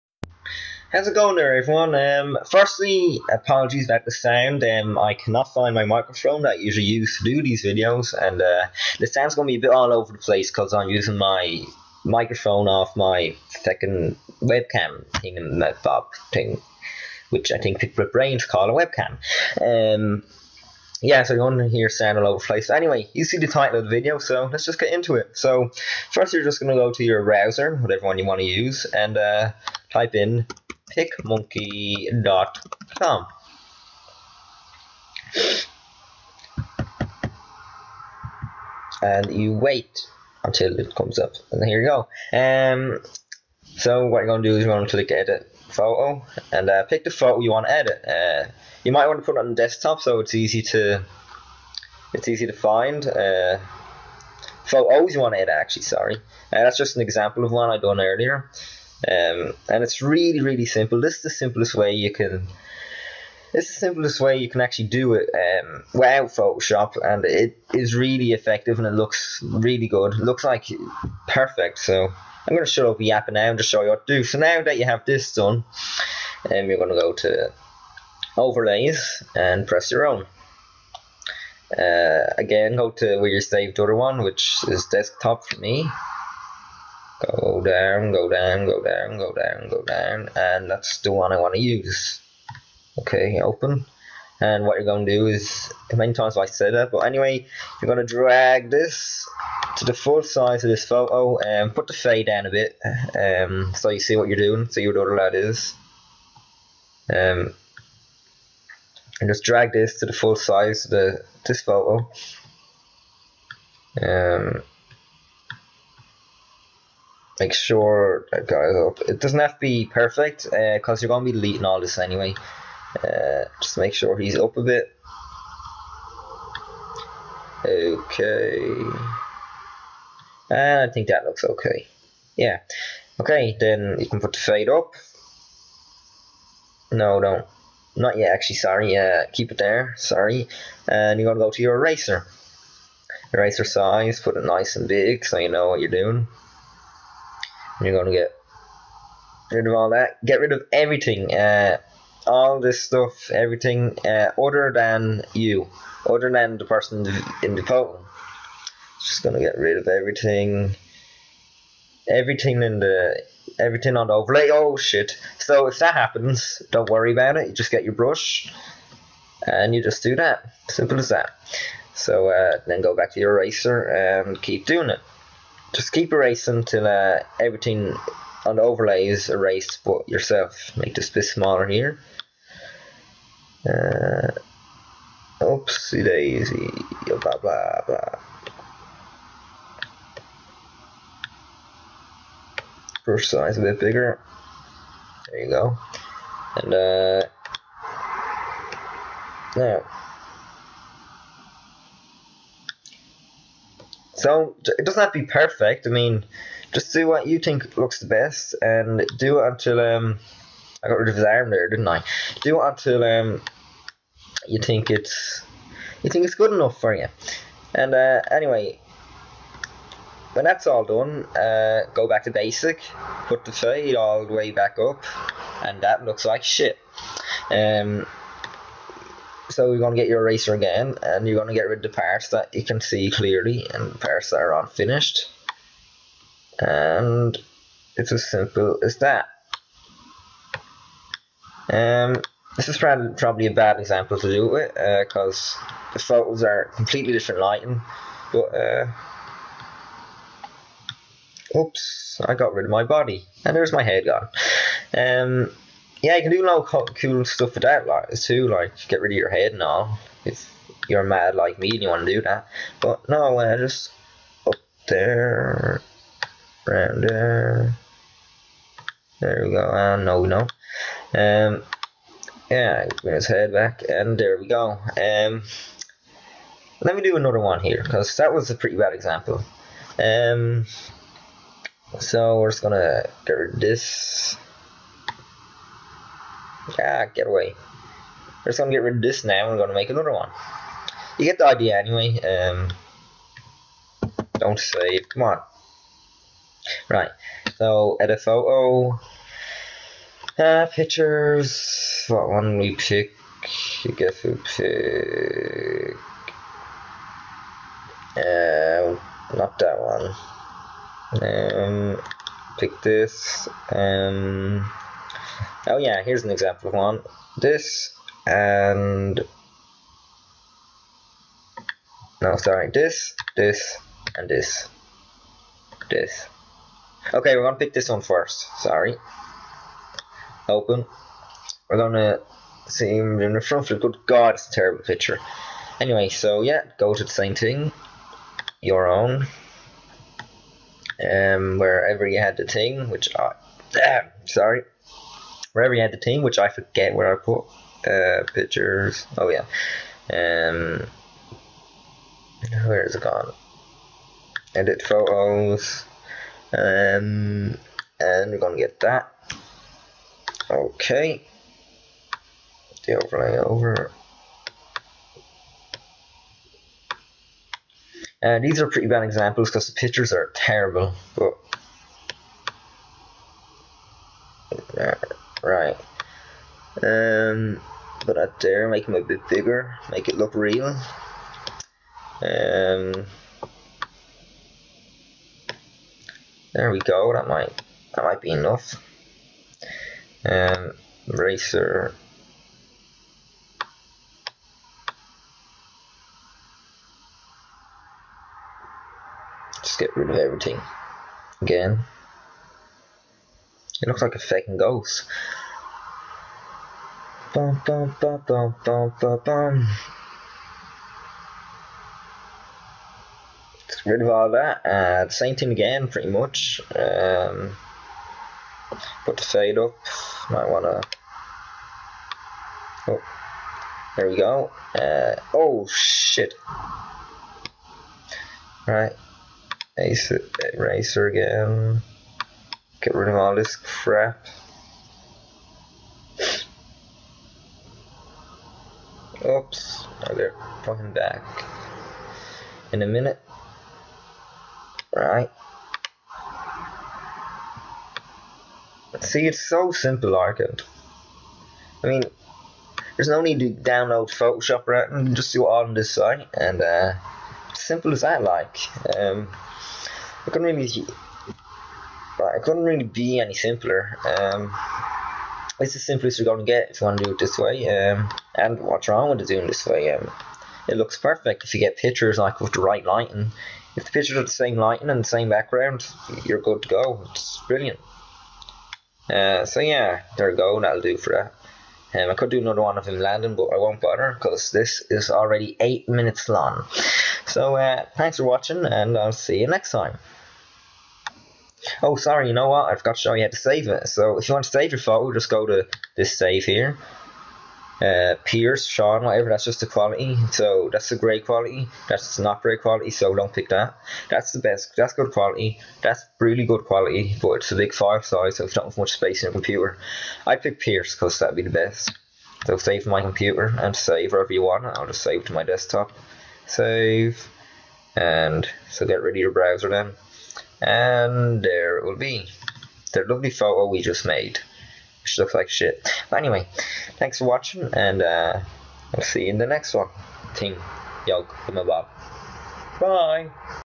Easiest way to clone yourself in photos WITHOUT PhotoShop (Really simple)
Don't wanna download a big ass file for PhotoShop? Don't wanna pay for PhotoShop? Is PhotoShop just too complicated for you? Well no worries! this video will show you the simplest and most effective way of cloning yourself or anything else you want using PicMonkey.com! (free website) :D